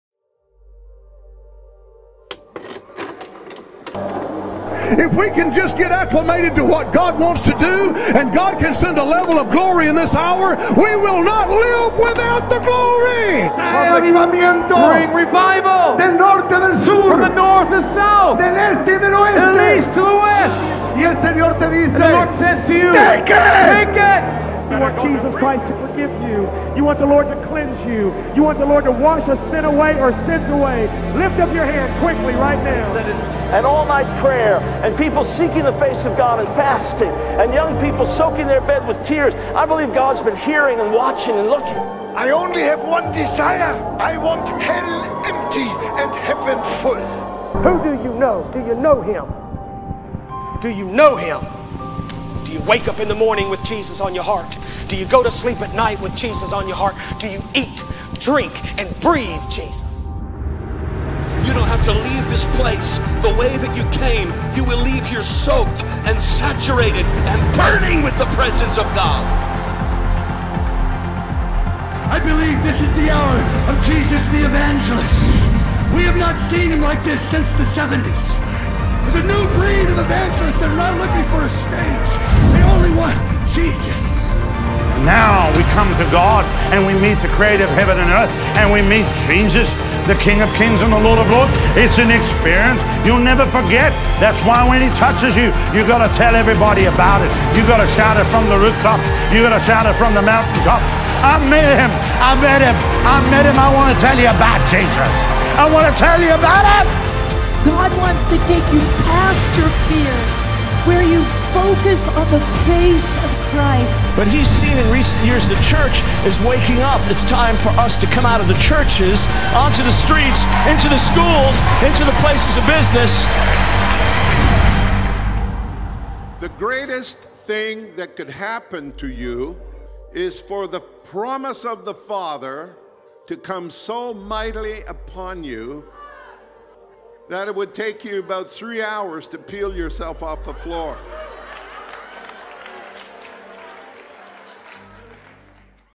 Light The Fire Again 2019 (Trailer)
“Light The Fire Again” is happening in Pensacola, Florida from 4 – 7 September 2019. Have a look at what’s in store!

(Video clip in English only)